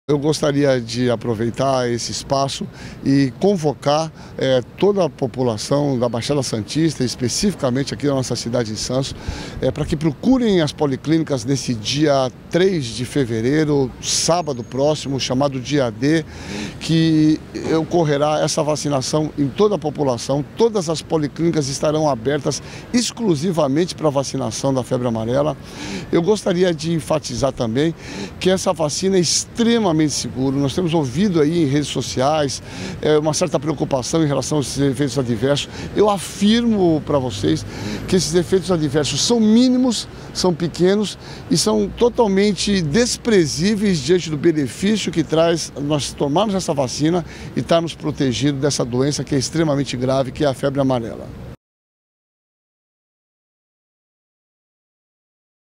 Sábado (3) é o 'Dia D' da campanha contra a febre amarela
Um dos mais renomados infectologista do país, Dr. Marcos Caseiro, tem um recado para o santista. Neste sábado (3), 22 policlínicas da Cidade estarão abertas, das 8 às 17hrs, especialmente para a campanha de vacinação contra a doença. E ele garante: a vacina é segura!